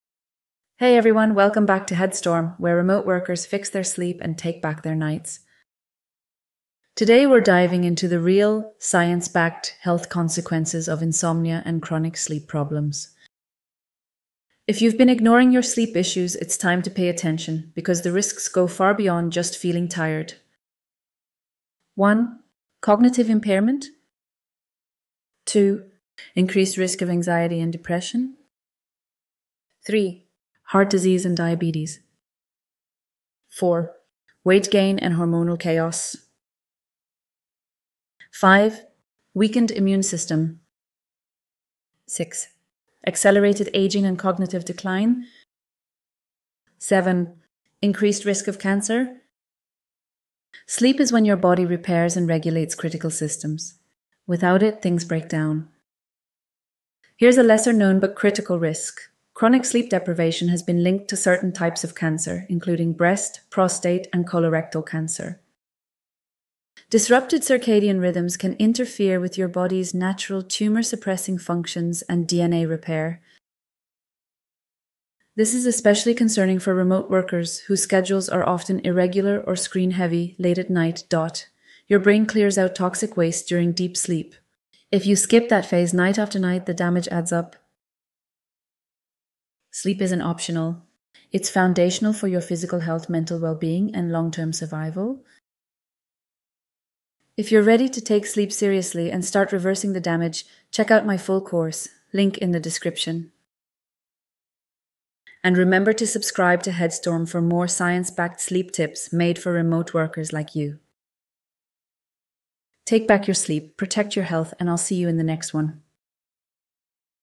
Hidden Dangers of Sleep Deprivation What Remote Workers Need to Know
You think you know what lack of sleep does to you? Think again.
Most people believe insomnia just makes you tired the next day. But the real health consequences go way deeper than that. And what science reveals about chronic sleep problems will shock you.

Here's what you'll discover in this video:

• The hidden damage happening to your body when you can't sleep
• Why your brain literally starts breaking down without proper rest
• The scary connection between sleep loss and serious health conditions
• How many hours of sleep you actually need (it's not what you think)
• The real reason some people can't fall asleep no matter what they try

I've spent months digging through the latest research on sleep and health. What I found changed everything I thought I knew about insomnia.

Your body doesn't just "get tired" when you don't sleep. Something much worse happens. Your immune system starts failing. Your memory begins to fade. And your risk for certain diseases shoots through the roof.

But here's the thing most doctors won't tell you about sleep problems...

The damage starts happening faster than you realize. Even one bad night affects your body in ways that last for days. And if you've been dealing with chronic insomnia? The effects stack up in dangerous ways.

I'm going to show you exactly what happens inside your body when you can't sleep. You'll see the real science behind why sleep matters so much. And you'll learn the warning signs that mean your sleep problems have gone too far.

This isn't about feeling groggy or drinking more coffee. This is about protecting your health before it's too late.

The research I'm sharing comes from top sleep labs around the world. These are the studies that changed how doctors think about insomnia. And once you see this data, you'll never ignore a sleepless night again.

If you've been putting off dealing with your sleep issues, this video will change your mind. The health risks are real. But so are the solutions.

Watch now to discover what chronic sleep loss really does to your body - and how to fix it before permanent damage sets in.